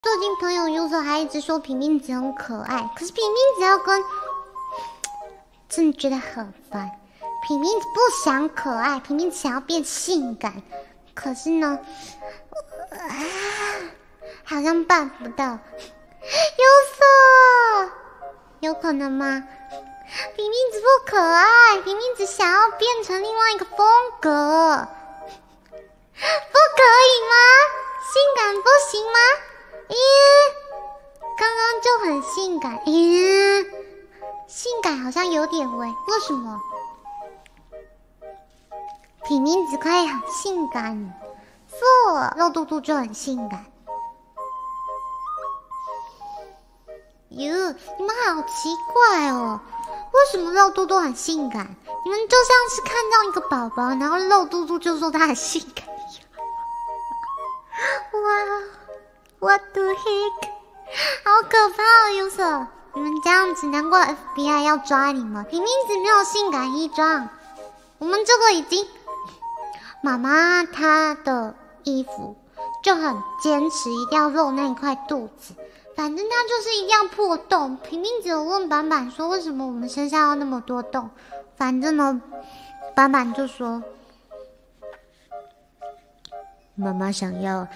【平平子精華】平平子抱怨朋友user 覺得朋友很煩? 順便說了新衣裝的製作花絮 透漏自己想變性感的小小心思?
性...性感嗎